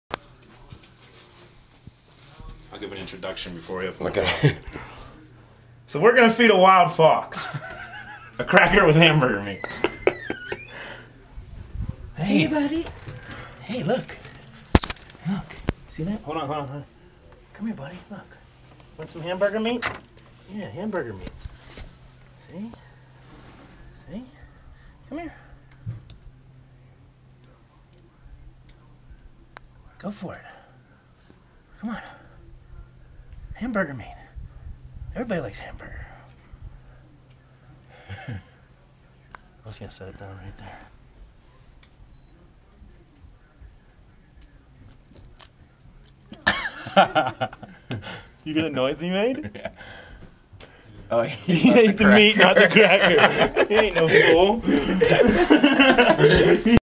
"Everybody Likes Hamburger!!" Feeding a wild fox in Winter Park, Colorado
This fox was just chillin after we fired up the grill, so I decided to hook him up for dedication!